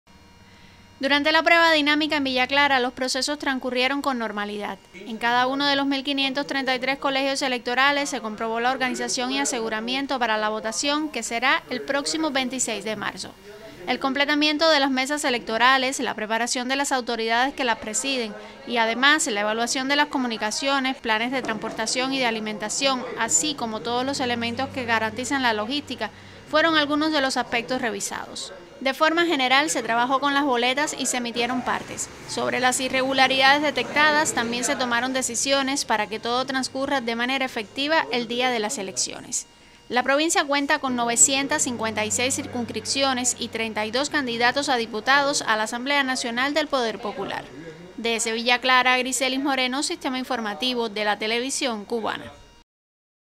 Realizan orueba dinamica en Villa Clara
Antes de cada jornada de elecciones Cuba siempre se realiza un prueba dinámica para detectar irregularidades y garantizar el correcto desarrollo del proceso, en Villa Clara la comprobación transcurrió con normalidad.